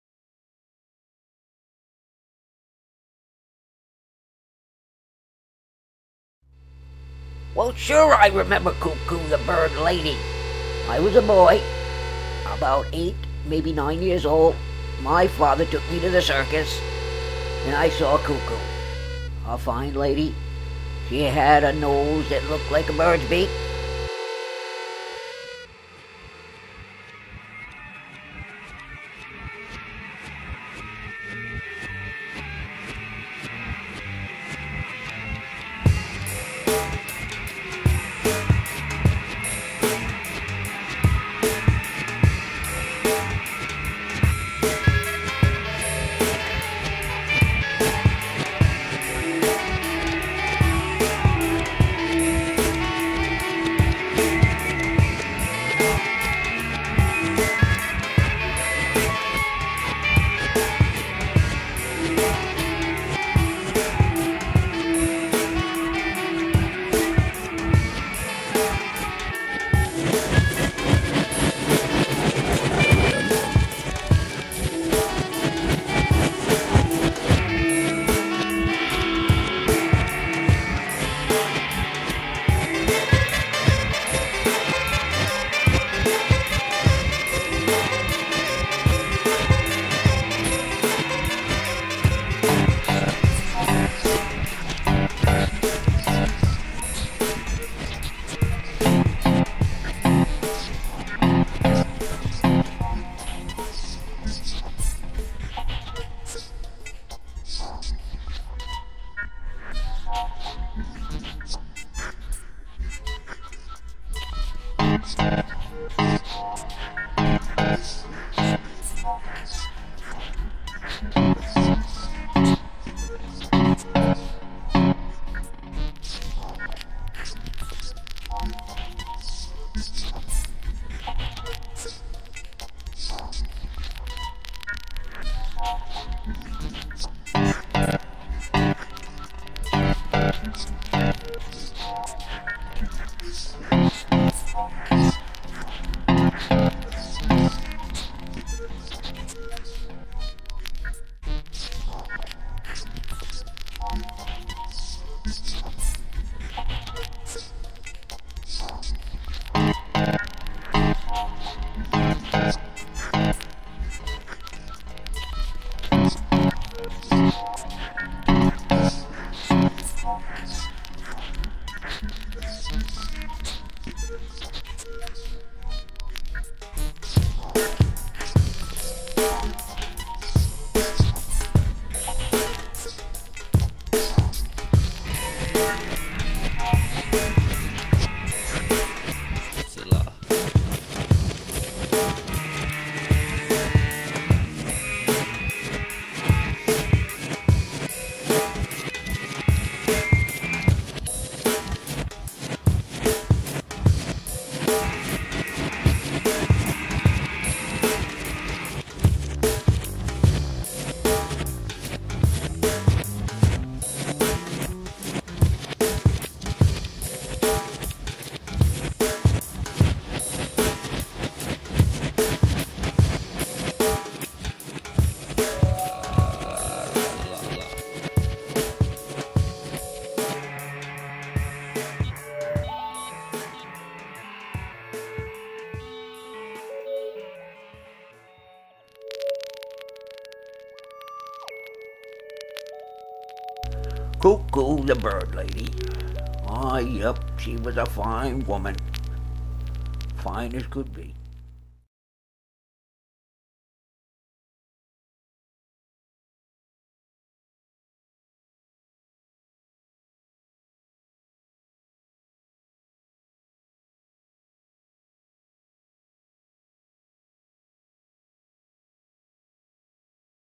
KOO KOO The Bird Lady
Portrait of Koo Koo the Bird Lady by Yannick Dangin Leconte.

See wikipedia article about Koo Koo the Bird Lady